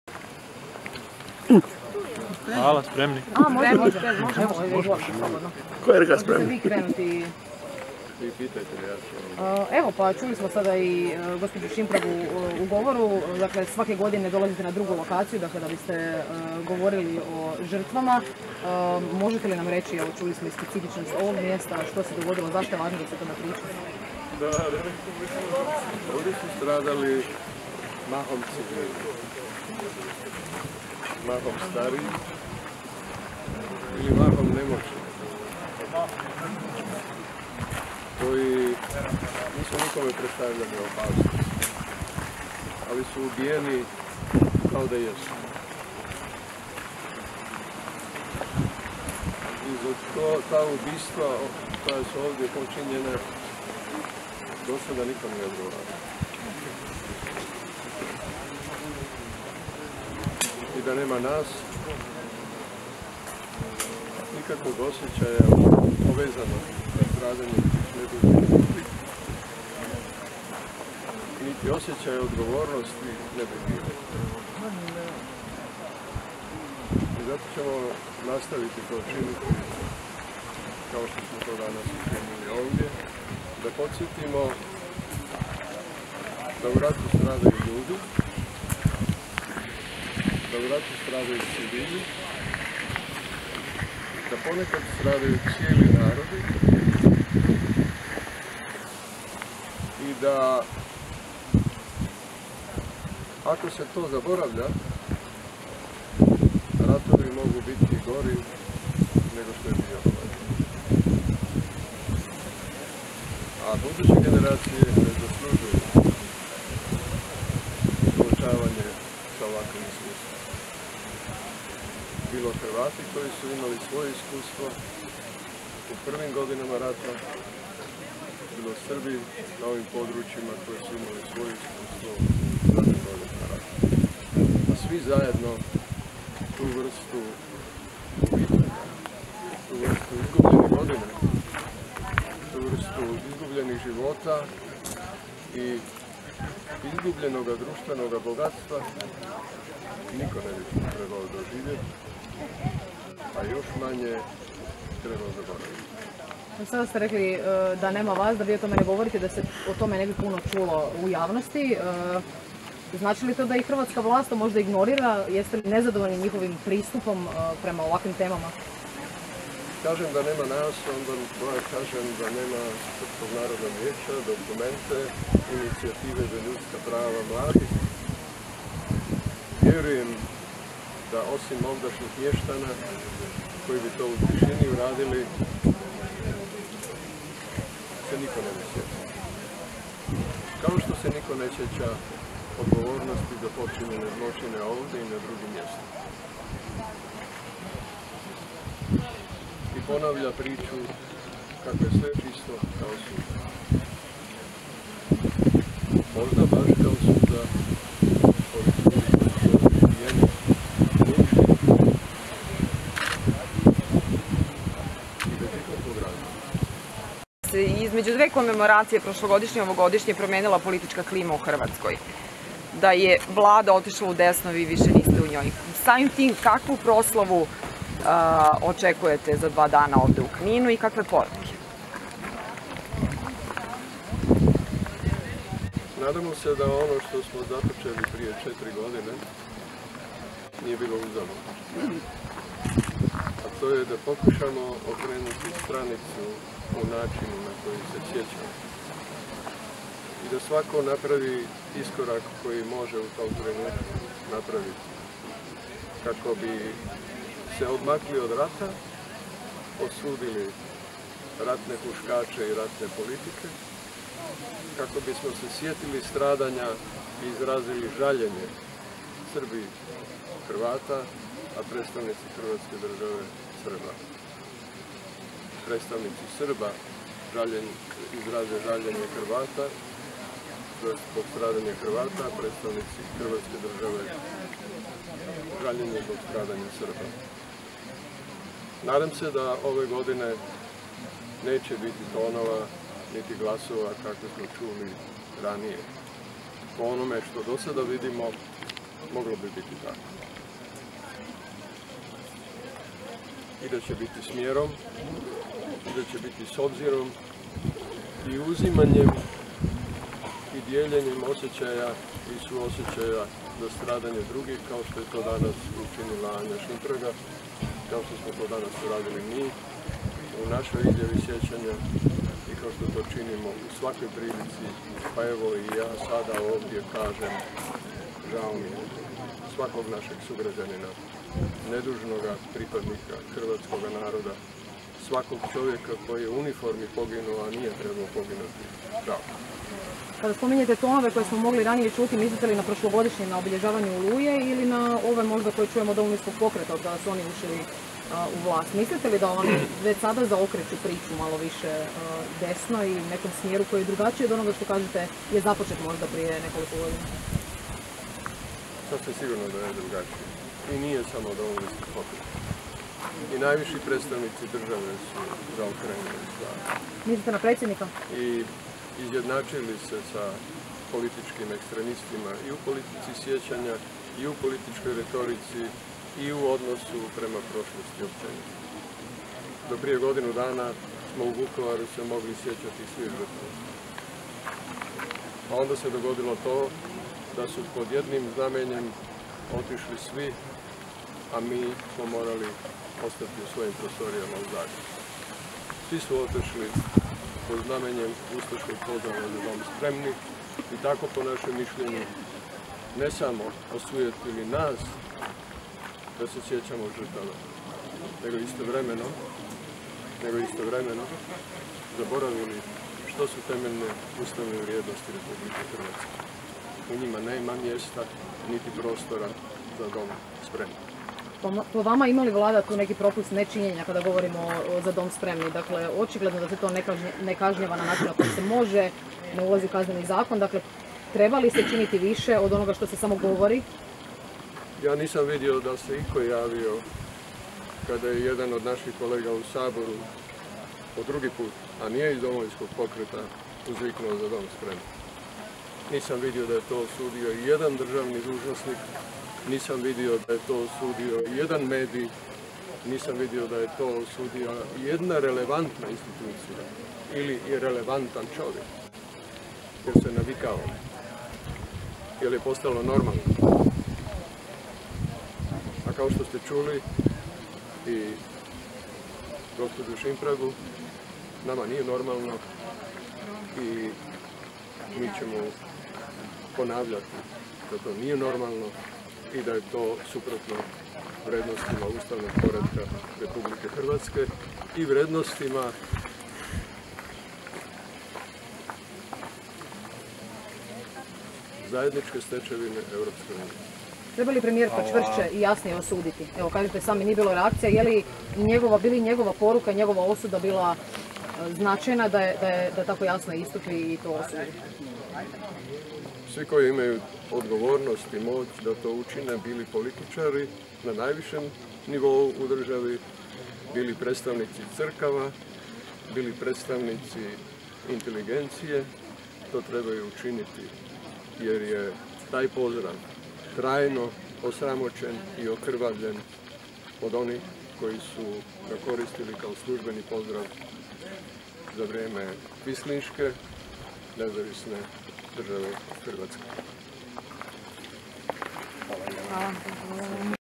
Milorad Pupovac na komemoraciji stradalima u VRO Oluji
Srpsko narodno vijeće organiziralo je komemoraciju posvećenu svima koji su izgubili živote, bližnje i domove tijekom VRO Oluja